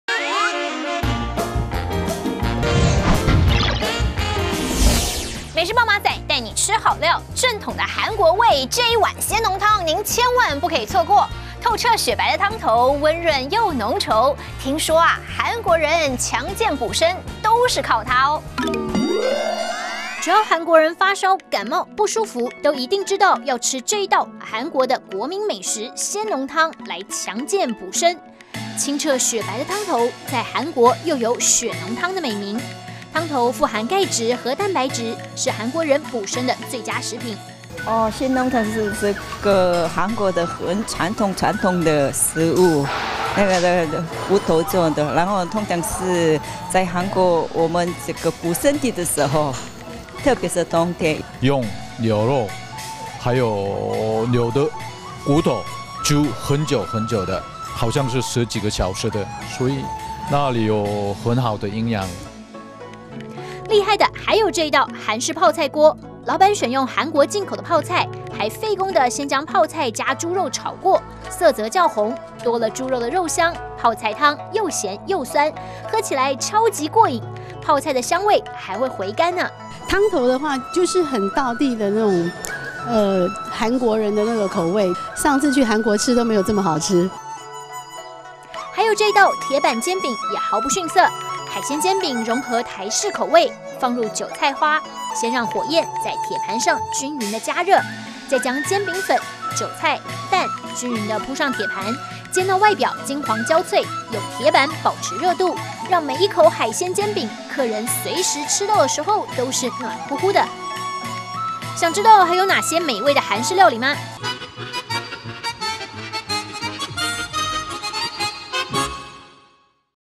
首爾鮮濃湯
強健補身的韓式牛骨湯，熬煮二十個小時以上，濃香又夠味！